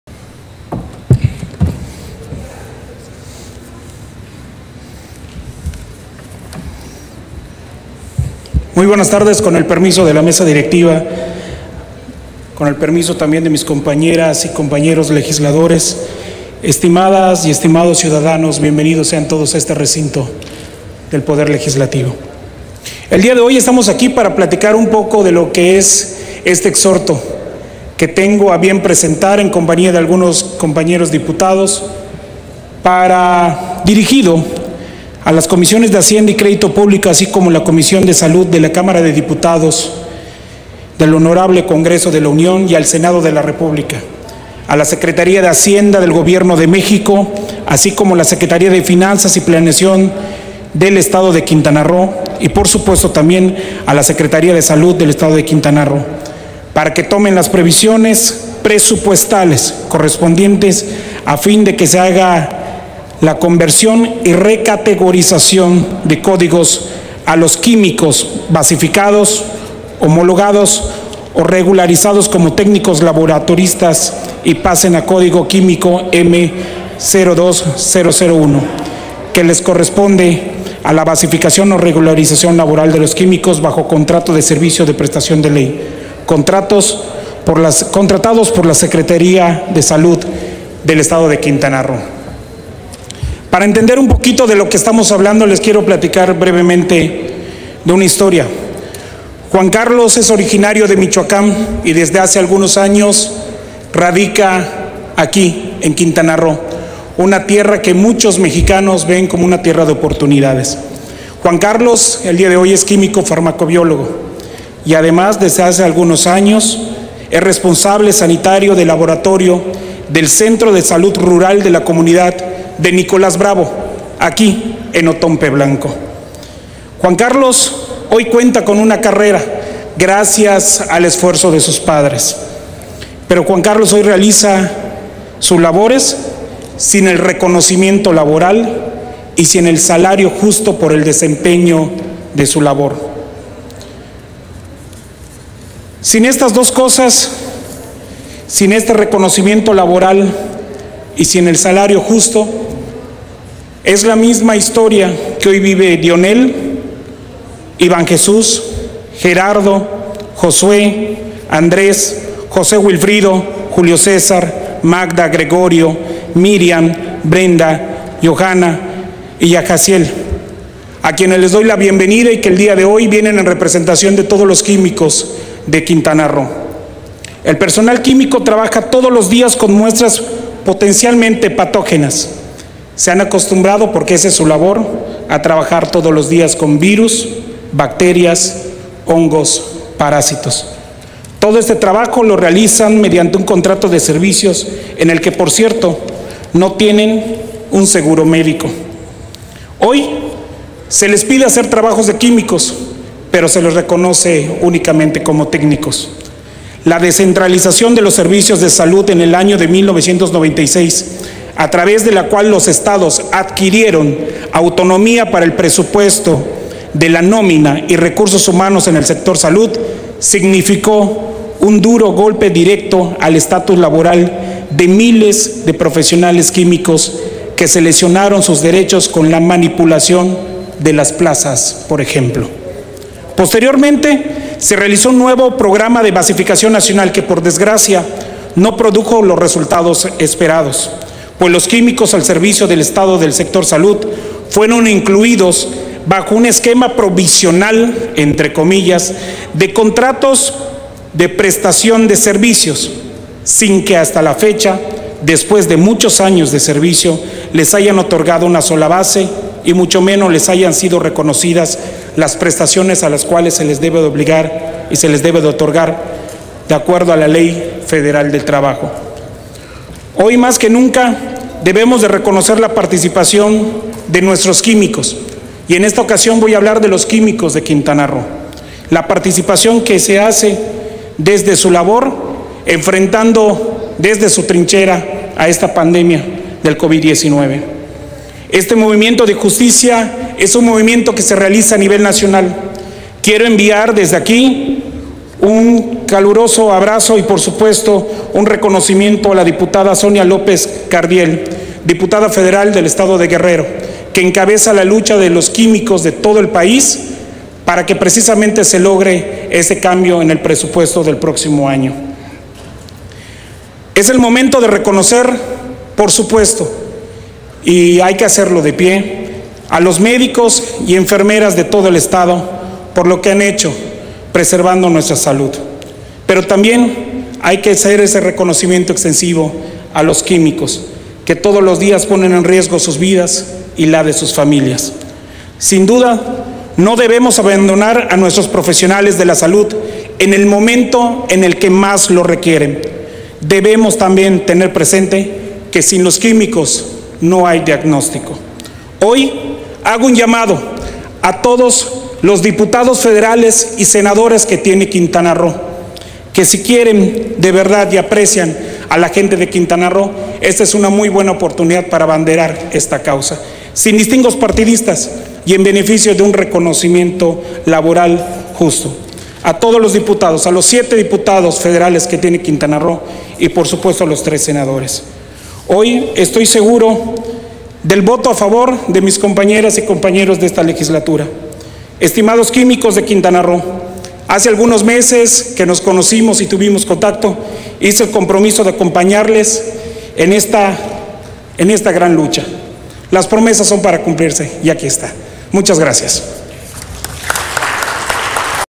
Intervención del Dip. Edgar Humberto Gasca Arceo. MORENA (15/Sep/2020)
Intervención del Diputado Edgar Humberto Gasca Arceo, de MORENA, en relación a la proposición con Punto de Acuerdo de obvia y urgente resolución, por medio de la cual, se exhorta respetuosamente a la Comisiones de Hacienda y Crédito Público, así como a la Comisión de Salud de la Cámara de Diputados del Honorable Congreso de la Unión y el Senado de la República; a la Secretaría de Hacienda del Gobierno de México, así como a la Secretaría de Finanzas y Planeación del Estado de Quintana Roo y a la Secretaría de Salud del Estado de Quintana Roo para que se tomen las previsiones presupuestales correspondientes a fin de que sea realizada la conversión y recategorización de códigos a las químicos basificados, homologados o regularizados como técnicos laboratoristas y pasen a código de "Químico A" M02001 que les corresponde a la basificación regularización laboral a los químicos bajo contrato de servicios y sin prestaciones de Ley, contratados por la Secretaría de Salud del Estado de Quintana Roo.